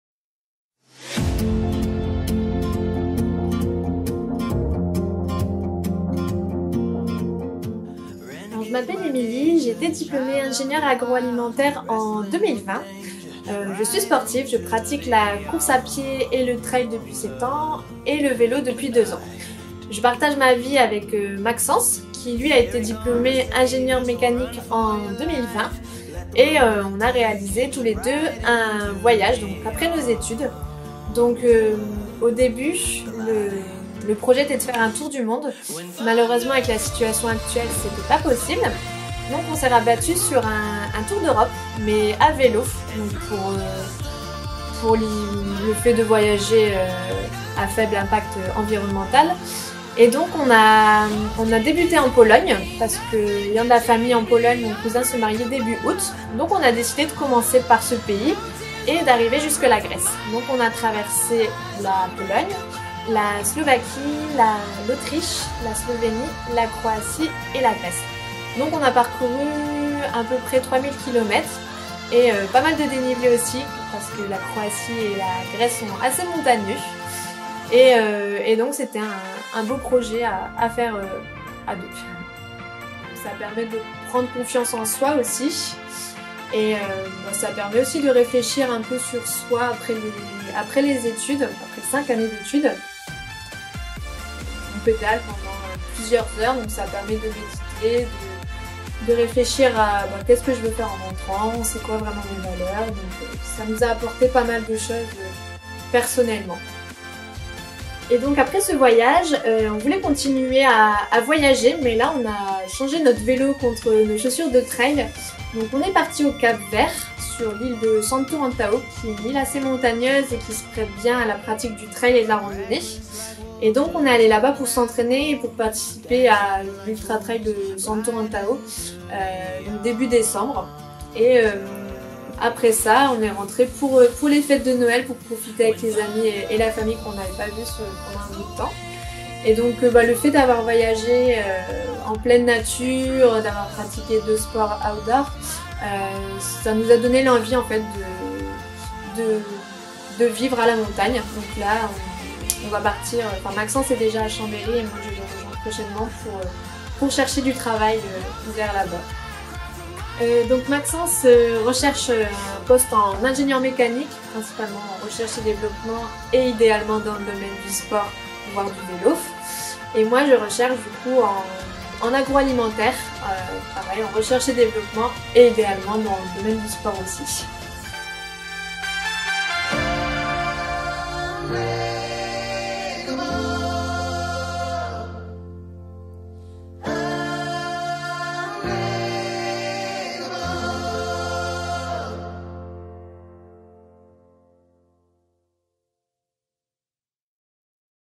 Tour d'Europe à vélo pour deux ingénieurs Polytech lillois
Fraichement diplômés, Emilie (génie biologique et alimentaire) et Maxence (mécanique) ont parcouru l'Europe à vélo... Prenez le nombre de kilomètres parcourus (3 000), multipliez par le nombre de pays traversés (7)… vous obtenez le nombre de leurs souvenirs européens !